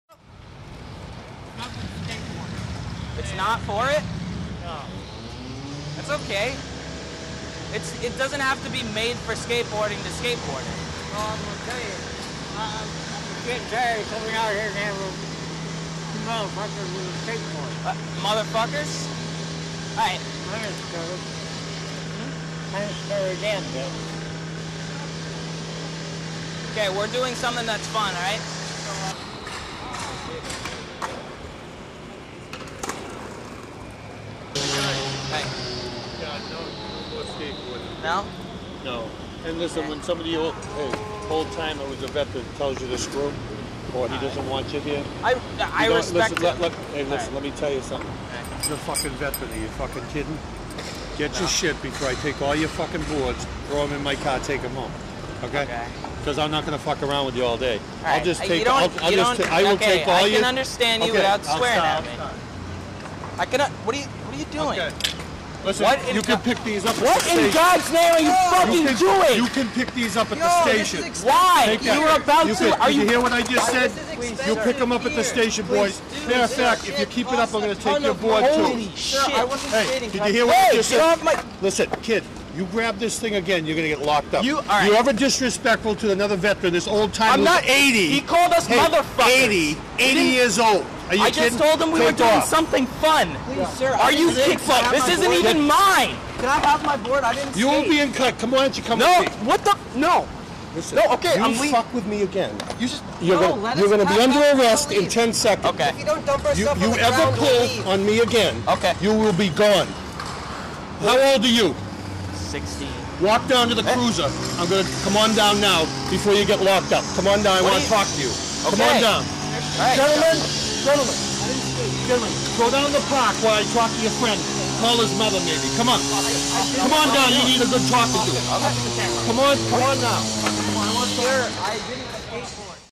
Short Tempered Cop Trashes Camera Equipment
So, basically we were just warming up at this spot in downtown boston. The footage looks and sounds like complete shit because I didn't really have time to set colors or audio. I kindof just took out the camera and this is what happened. This elderly man from the homeless veteran's shelter didn't really want us skating. I respectfully told him that we were just having fun. But, he didn't buy it. He hobbles over to a nearby police man, and apparently told the police man that we insulted him and threatened to hurt him. As you can clearly see, we did nothing of the sort. So this police officer starts running his fowl mouth, and unreasonably swearing in a public area. So when I ask him to stop swearing and not make a huge deal out of hit, HE FLIPS. See for yourself. I eventually talked him out of taking out boards. Filmed by Austin Delin. Sony Vx1000/MK1.